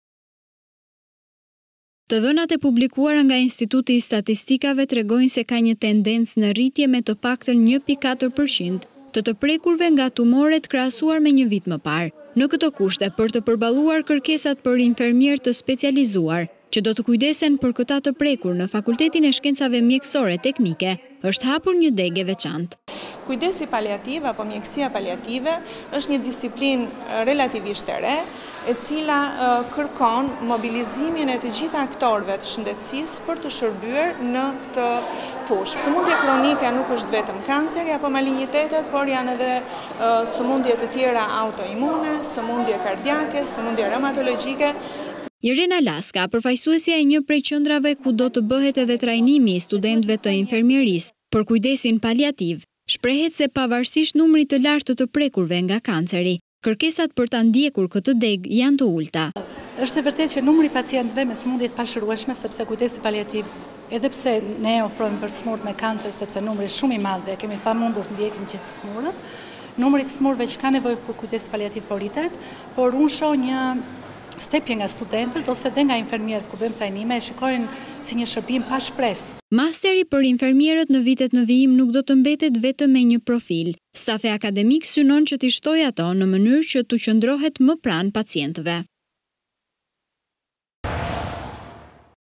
Ora News - Kujdesi paliativ, degë e re në Masterin e infermierisë
Rritja e rasteve të të prekurve nga tumoret është rritur me 1.4 përqind më shumë se një vit më parë. Fakulteti i Shkencave Mjekësore Teknike për herë të parë këtë vit akademik ka hapur profilin e kujdesit paliativ për studentët e ciklit master të infermierisë.

Të dhënat e publikuara nga Instituti i Statistikave tregojnë se ka një tendencë në rritje me të paktën 1.4 për qind të të prekurve nga tumoret krahasuar me një vit më parë. Në këto kushte, për të përballuar kërkesat për infermierë të specializuar që do të kujdesen për këta të prekur në Fakultetin e Shkencave Mjekësore dhe Teknike është hapur një degë e veçantë.

"Mjekësia paliative është një disiplinë e re që kërkon mobilizimin e të gjithë aktorëve të shëndetësisë për të shërbyer në këtë fushë. Sëmundje kronike nuk është vetëm kanceri apo malinjitetet, por janë sëmundjet e tjera autoimune, sëmundje kardiake, sëmundje reumatologjike".

Irena Laska, përfaqësuesja e një prej qendrave ku do të bëhet edhe trajnimi i studentëve të infermierisë për kujdesin paliativ, shprehet se pavarësisht numrit të lartë të të prekurve nga kanceri, kërkesat për të ndjekur këtë degë janë të ulëta.

Irena Laska: Është e vërtetë që numri i pacientëve me sëmundje të pashërueshme po rritet. Unë shoh një stepje nga studentët, por edhe nga infermierët. E shikojnë si një shërbim pa shpresë.

Masteri i Infermierisë në vitet në vijim nuk do të mbetet vetëm me një profilizim, stafi akademik synon ti shtojë ato në mënyrë që tu qëndrohet më shumë pranë pacientëve.